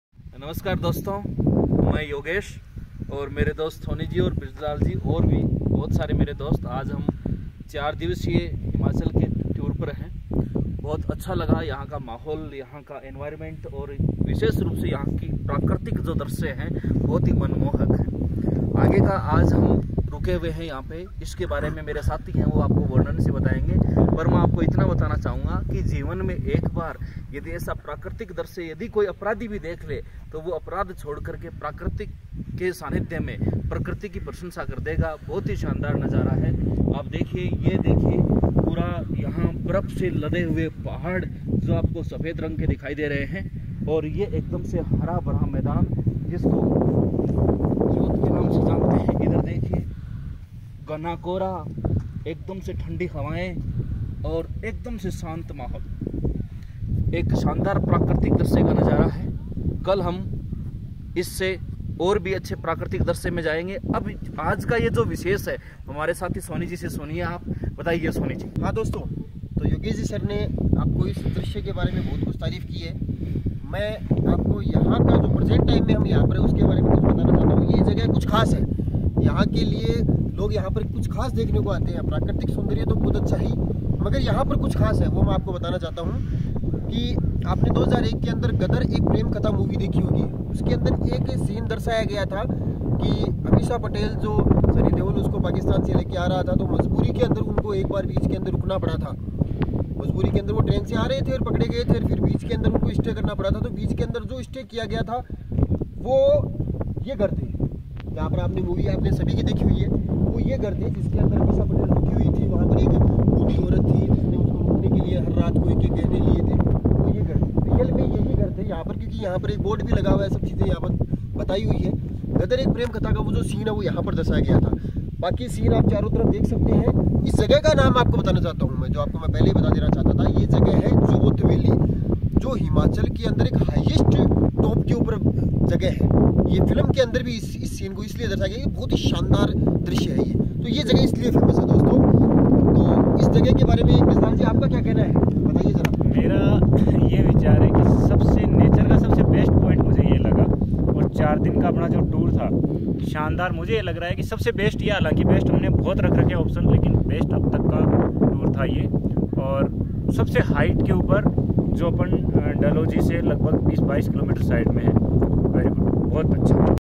Himachal tour, dalhousie, Khajjiar, Mcleodganj, area of Gadar film, shooting jyoti velly, tour .
दोस्तों के साथ हिमाचल की वादियों का भ्रमण,
Best visiting place in Himachal
Best planning for Himachal tour